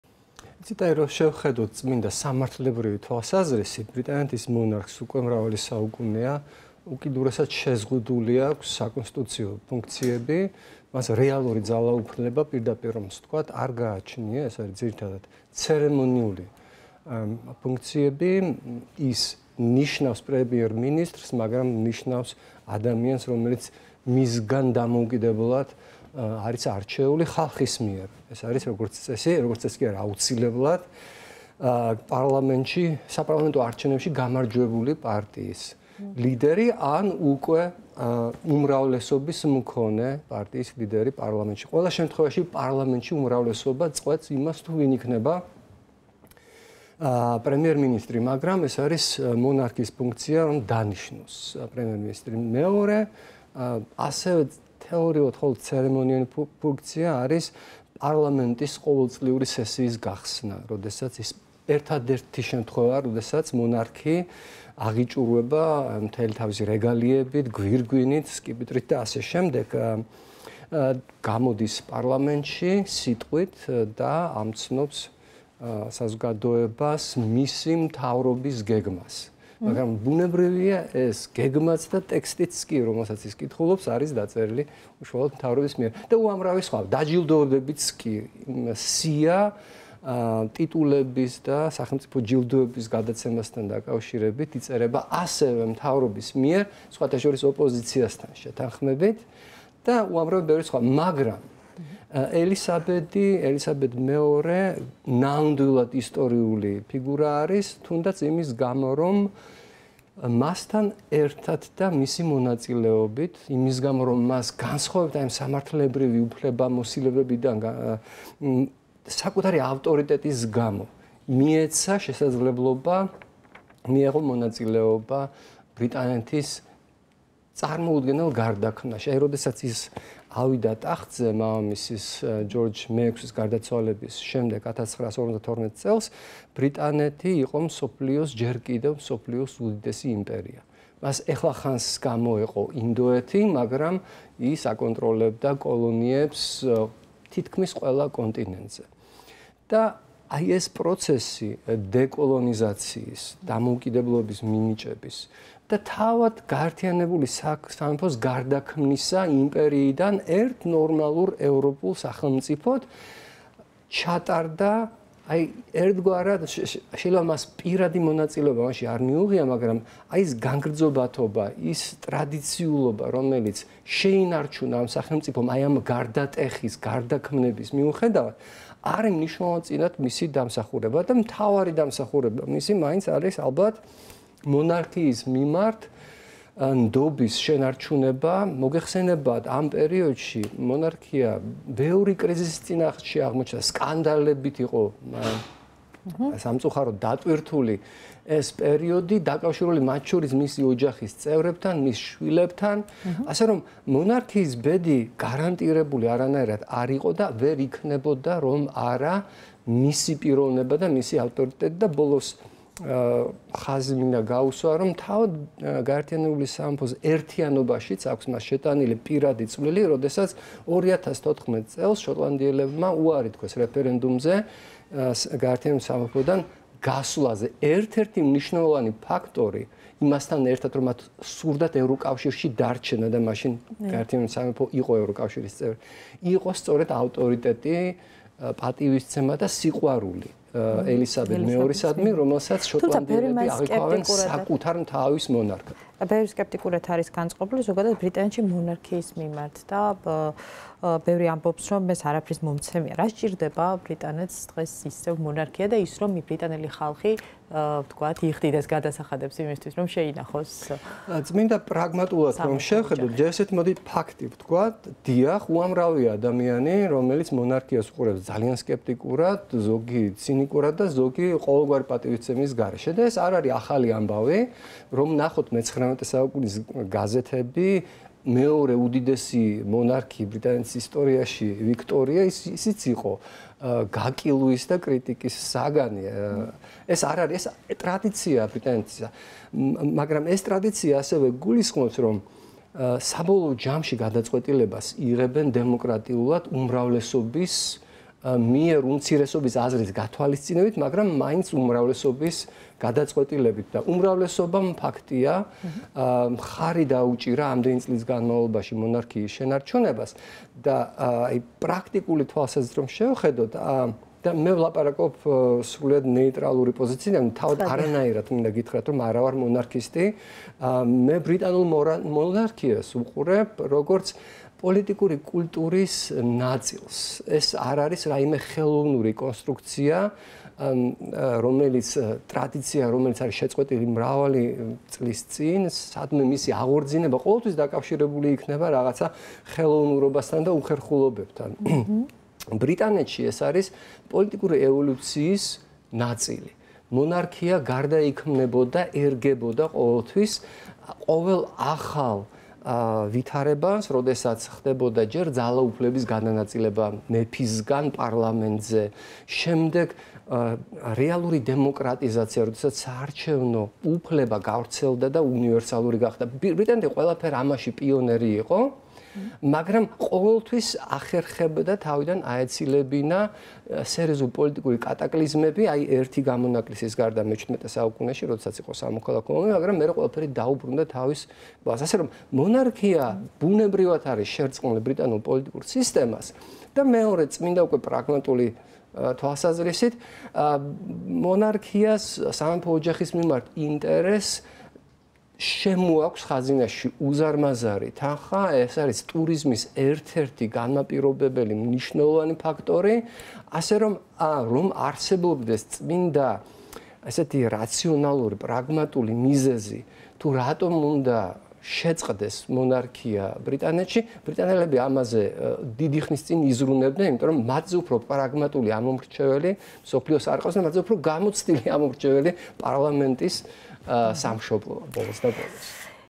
დედოფლის ეპოქა დასრულდა - საქართველოს ყოფილ ელჩი ბრიტანეთში, გიორგი ბადრიძე
დედოფალი ელისაბედ მეორე გარდაიცვალა. ის იყო სამეფო ტახტზე ყველაზე ხანგრძლივად მყოფი მონარქი ბრიტანეთის ისტორიაში. როდესაც ელისაბედი დედოფალი გახდა და თავზე გვირგვინი დაიდგა, სამყარო მაშინ სულ სხვანაირი იყო. მას შემდეგ დედოფლის თვალწინ იცვლებოდა მთელი მსოფლიო და ბრიტანეთიც.

ინტერვიუ საქართველოს ყოფილ ელჩთან ბრიტანეთში, გიორგი ბადრიძესთან